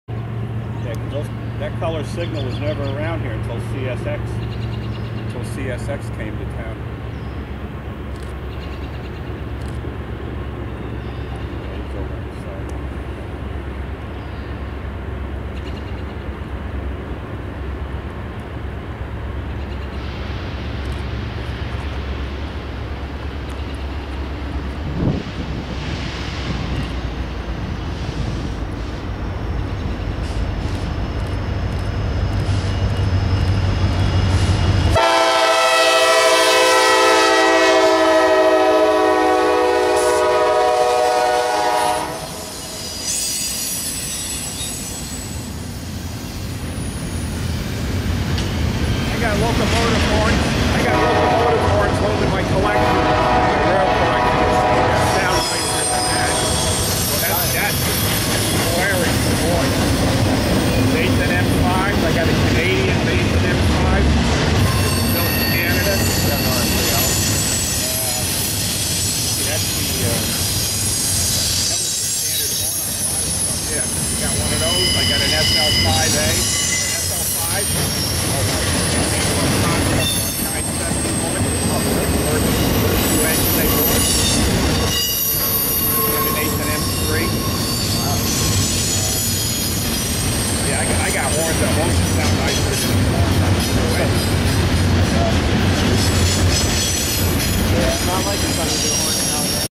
YN3B SD70MAC Leads CSX Q114 At CP 78 West Park NY
Video taken April 18th 2015.  Video by Ethan Sansoni.

CSX EMD SD70MAC 4825
CSX GE AC44CW 250
CSX GE ES44DC 5419